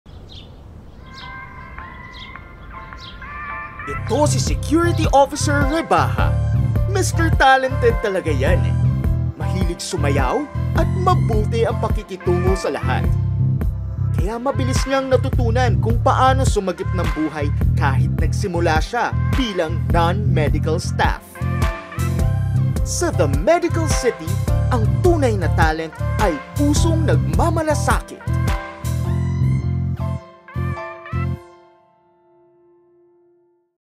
EveryBeatMatters - Mr. Talented
Ang TunayNaTalent, ay pusong nagmamalasakit. Ngayong Pasko, handa ang buong TMC Community na patuloy magbigay ng serbisyo.

Kung ikaw ay nakakaramdam ng sintomas ng atake ng puso, madaliang dumiretso sa mga ER ng The Medical City Ortigas, South Luzon, Clark, Pangasinan, at Iloilo